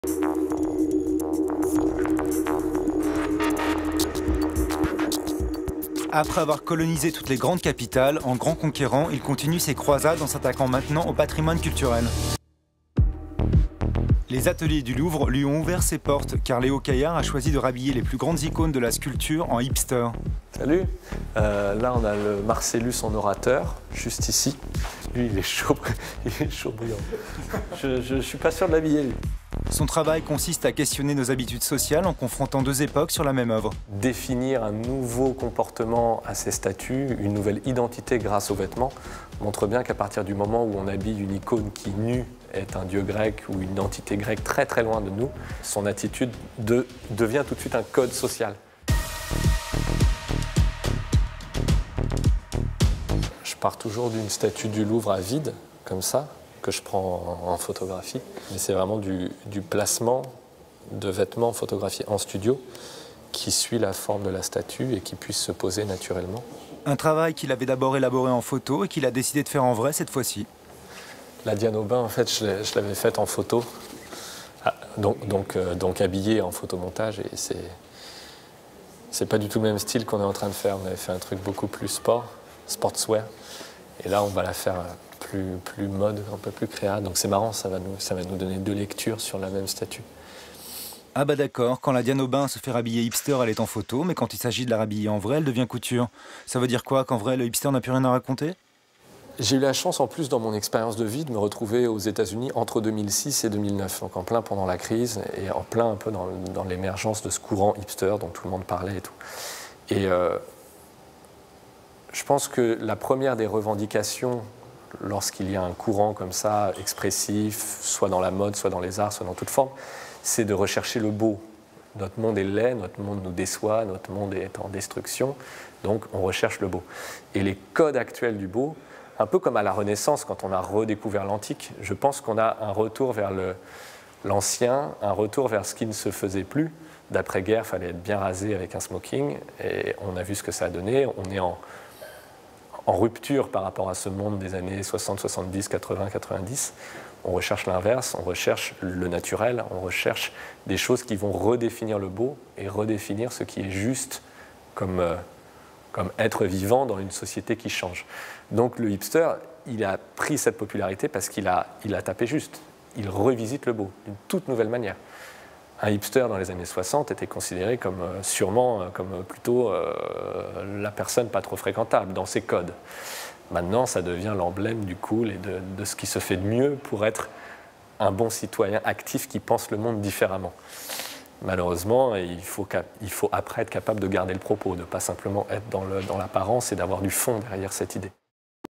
Léo Caillard habille les moulages de l'atelier de la Rmn-GP
Le jeune photographe Léo Caillard se passionne pour le mélange des époques. Une visite au Louvre, dans la salle Richelieu lui inspire "Hipster in Stone", un habillage des statues antiques selon les codes des "jeunes branchés".
C'est à l'atelier de moulage de la Réunion des musées nationaux-Grand Palais que l'artiste est venu exercer ses talents… et en 3D, s'il vous plait !
A voir dans un documentaire sur Paris Première le 14 mars à 10h.

Produit par Mademoiselle Agnes / Lalala productions
Réalisé par Sébastien Haddouk